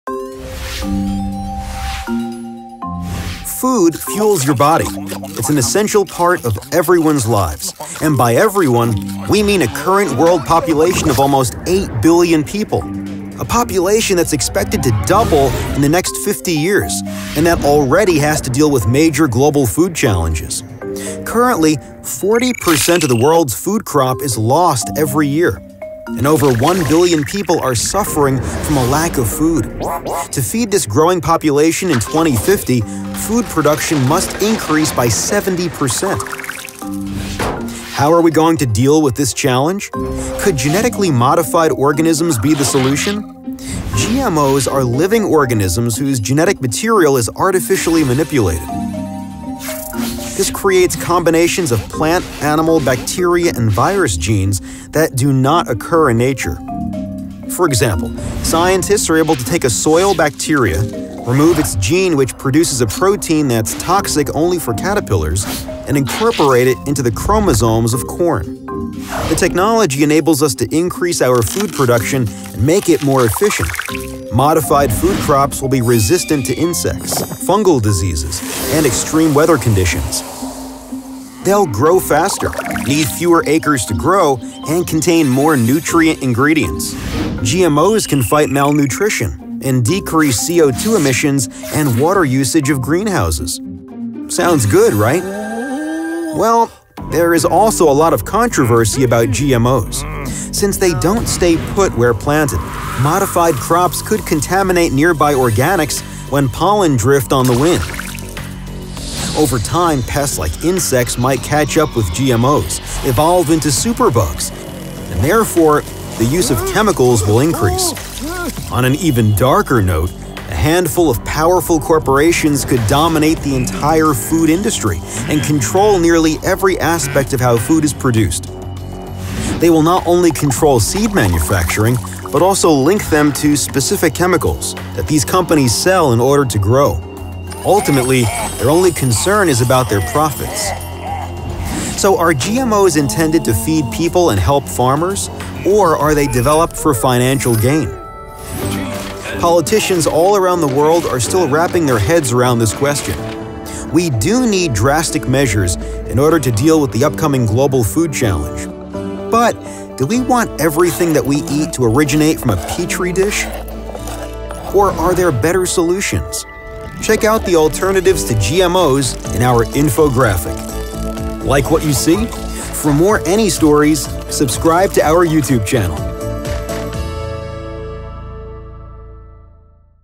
Genetically Modified Organisms (GMO): the future? | AnyStory made by Cooler Media
In this video you'll learn about Genetically Modified Organisms (GMOs). Are GMO's the future?

They’re more common in our food than you might think: Genetically Modified Organisms. Plants whose DNA has been genetically manipulated, seem to be the solution to the growing demand for food in our world, but are GMOs safe?

to download the infographic.

Timestamps:
00:00 Introduction
00:20 Could Genetically Modified Organisms be a solution to modern day food challenges?
00:40 What are genetically Modified Organisms?
01:38 Controversy around GMO's